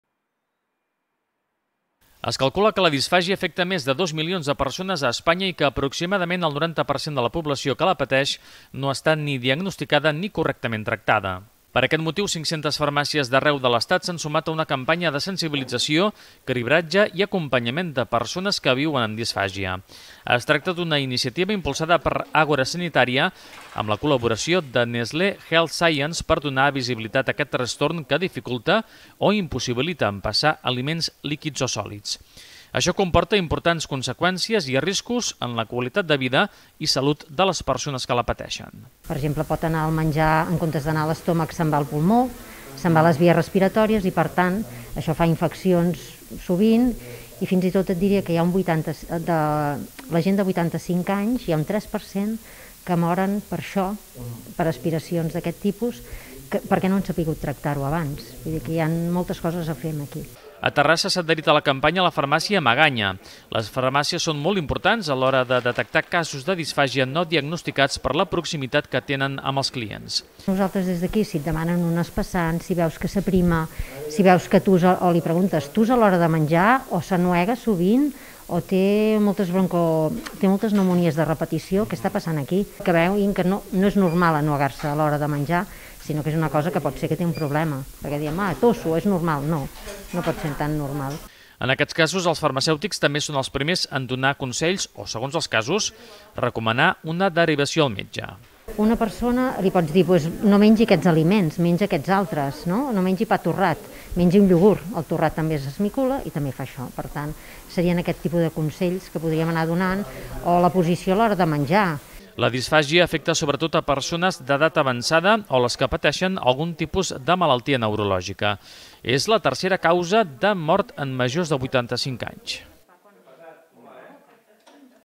Campanya de les farmàcies per a sensibilitzar, cribar i acompanyar persones amb disfàgia
Es calcula que la disfàgia afecta a més de dos milions de persones a Espanya i que, aproximadament, el 90% de la població que la pateix, no està ni diagnosticada, ni correctament tractada. Per aquest motiu 500 farmàcies d’arreu de l’estat s’han sumat a una campanya  de sensibilització, cribratge i acompanyament de persones que viuen amb disfàgia Més informació a